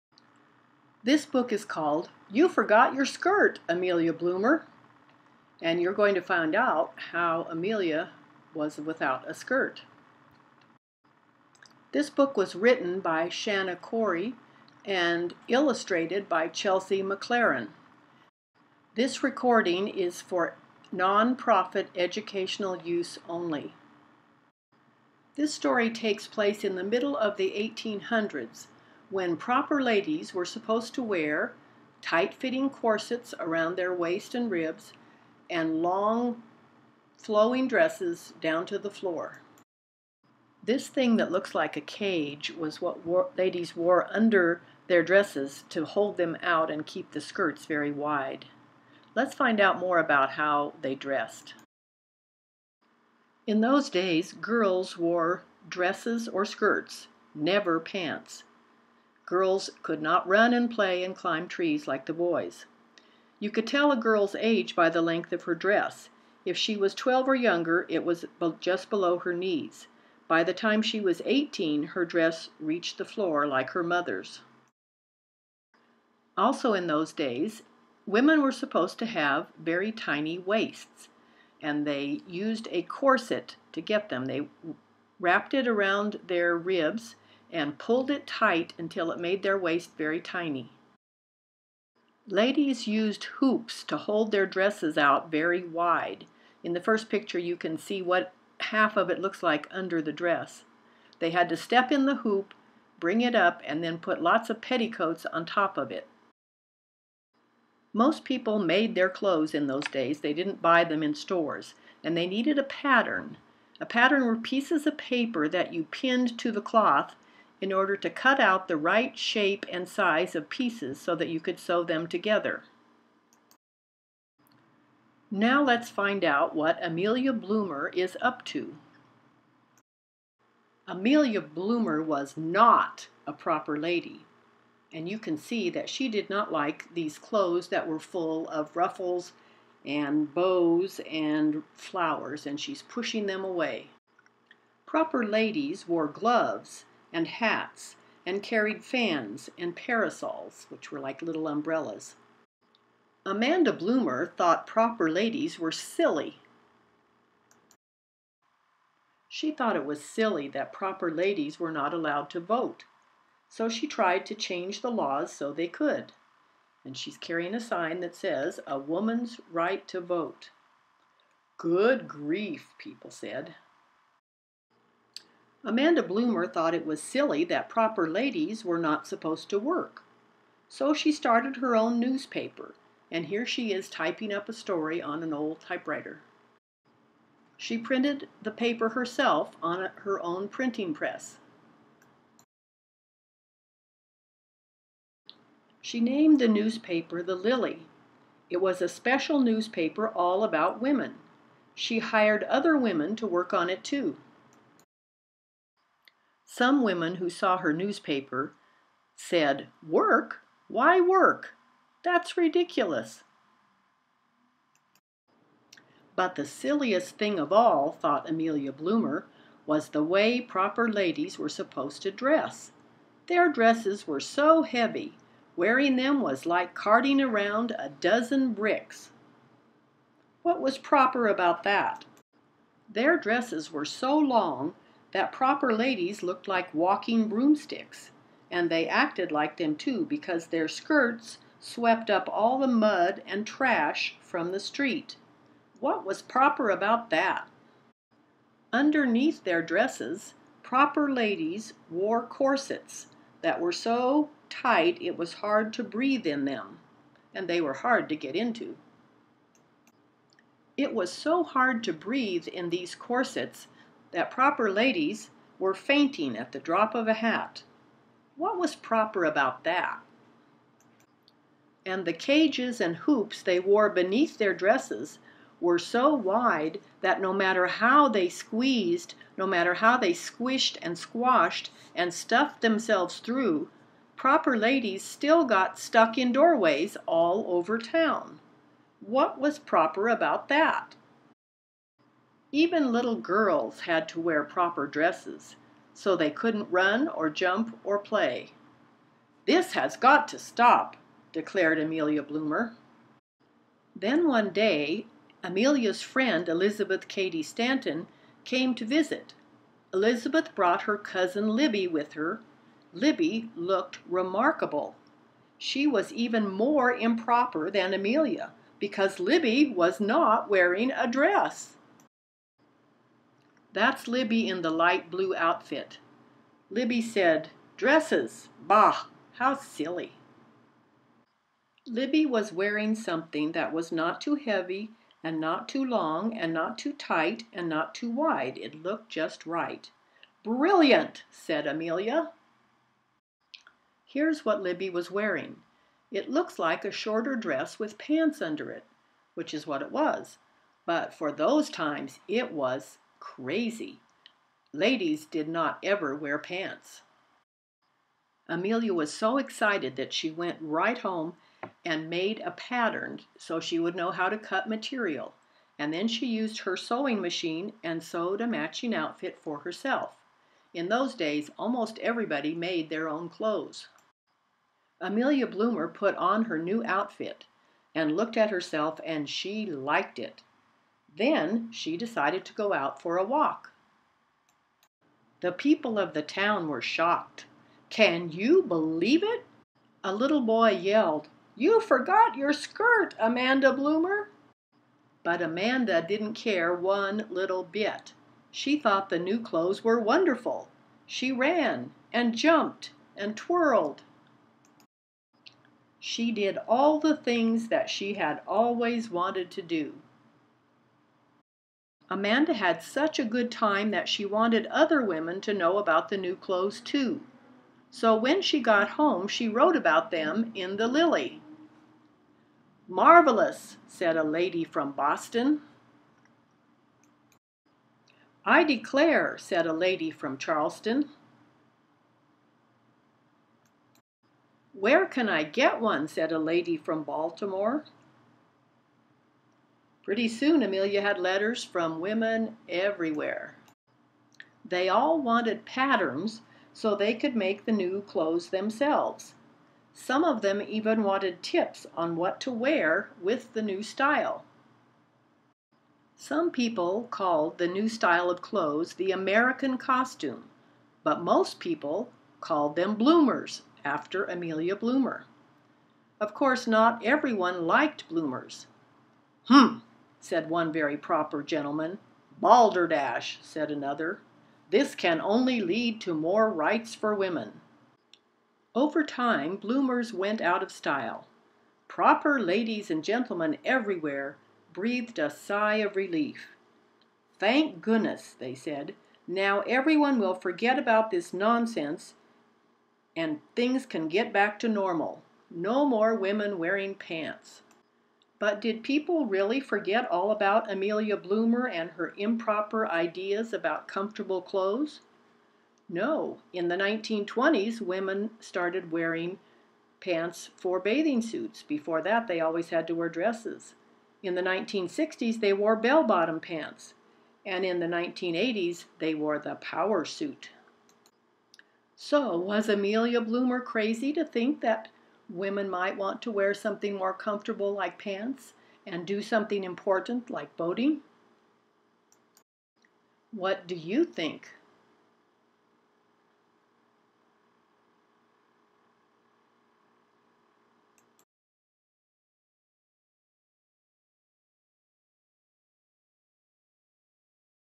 You Forgot Your Skirt, Amelia Bloomer! Read aloud book by Shana Corey
You Forgot Your Skirt, Amelia Bloomer  (dress reform and the vote)
READ ALOUD video  -  for non-profit educational use only
Read by Kathleen DeHerrera
by Shana Corey, illustrated by Chesley McLaren, 2000, Scholastic Press

level:   read-aloud and discuss – late 2nd-5th grade

read-aloud time: about 9 minutes

36 picture book pages  (colorful drawings)
+ one page Author’s Note with details on Amelia Bloomer and bloomers

summary:
--tells of 1800s suffragist, Amelia Bloomer, who did not want to be a proper lady including not voting
--shows problems with fashions of the day, like long, heavy skirts, corsets, hoops
--shows a visit from Libby in a totally new kind of outfit (pants under a skirt)
--reveals reactions, pro and con, of others to the new outfit
--demonstrates what Amelia could do in the new outfit
--tells how Amelia publicized the outfit and its pattern, so it was named “Bloomers”

vocabulary to pre-teach or explain as you go:
 “proper lady”   words to express reactions:
 corset     Marvelous!
 hoops     Shocking!
 pattern (for dressmaking)  I declare!
 improper    Balderdash!

intro:  Describe how, in a long ago time, women and girls wore only dresses and skirts.  They were supposed to stay at home, not have jobs, and were not allowed to vote.

questions:

1.  How was Amelia different from proper ladies? (wanted to vote, have a job, dress differently)

2.  What was wrong with the clothes ladies were supposed to wear? (too heavy, too long, drug in dirt, too tight)

3.  Why couldn’t little girls run or climb trees? (had to dress like the grown-up ladies)

4.  What new kind of clothes did Libby show Amelia? (pants, but covered by a shorter skirt)